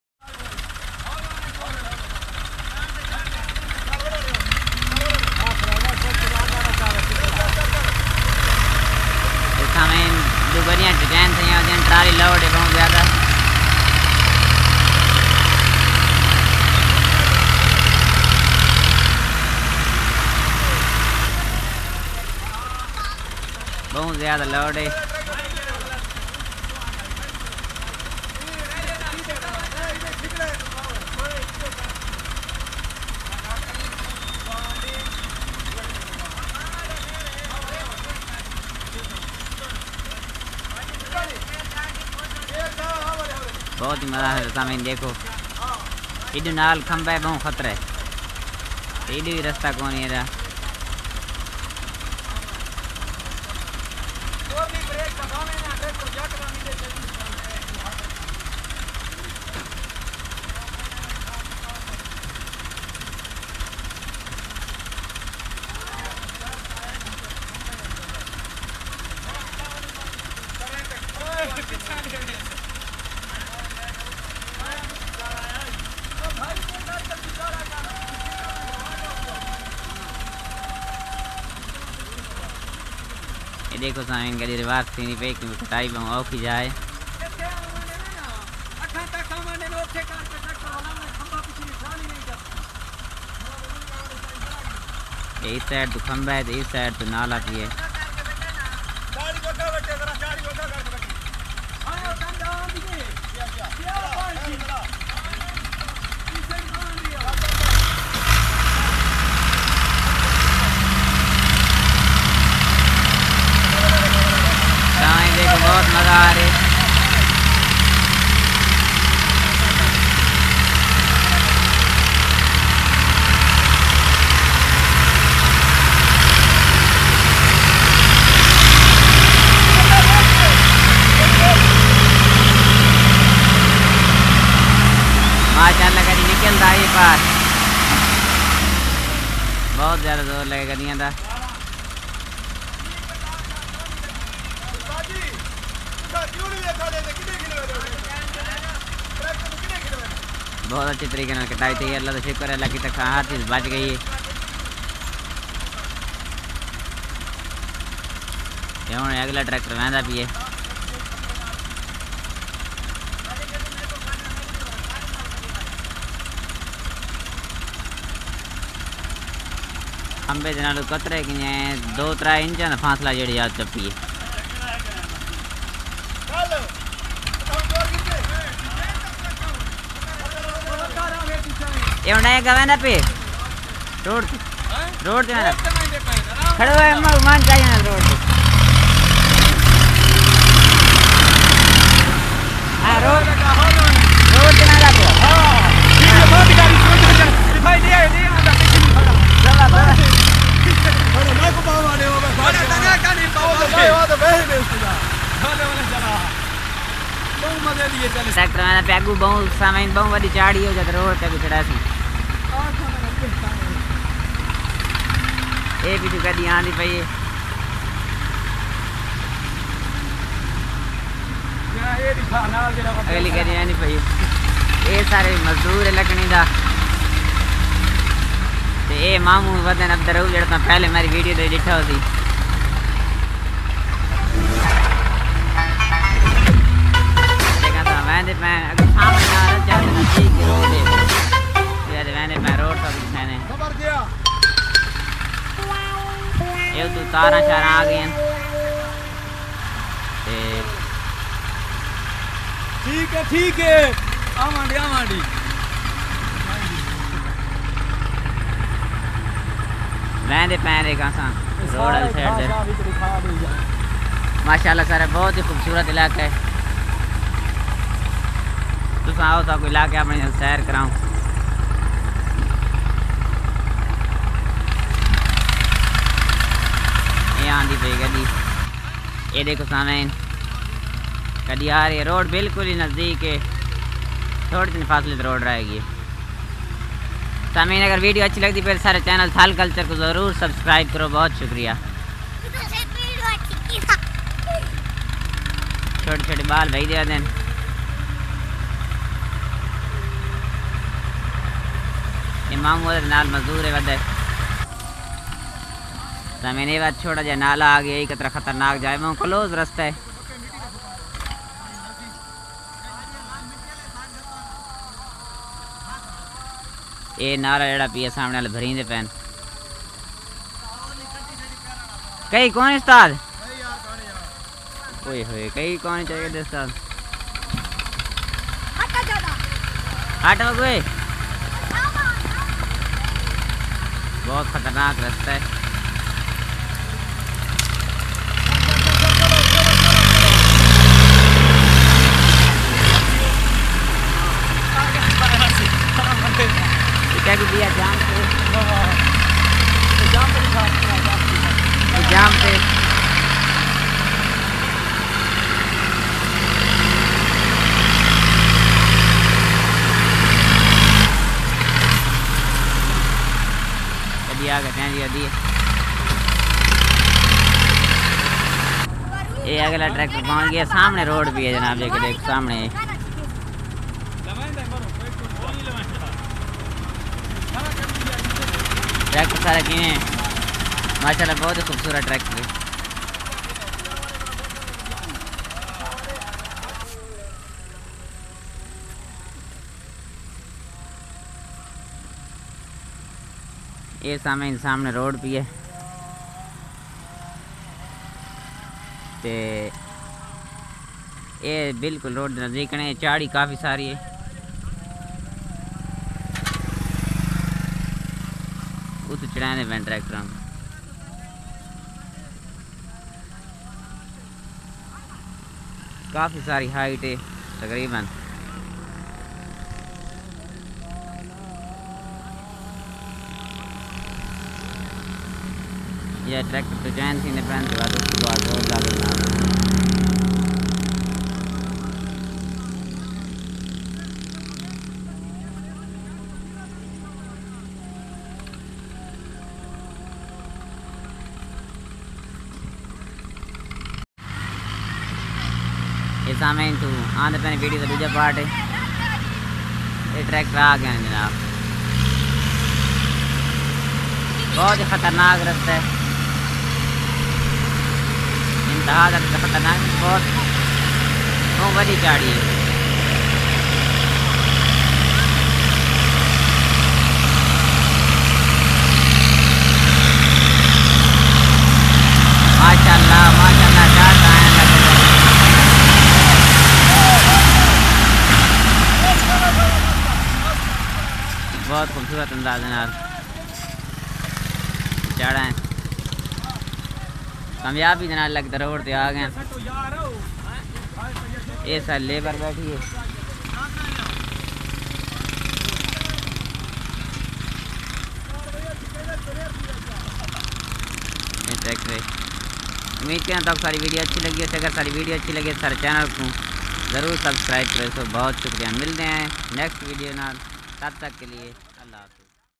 Tractor Driving /Lod tractor driving /Hard drive places
Is video main Thal ka elaqay main jo shandar triqa e driving hy us ko ujagar kia ja ra hy
Admin M Mudasir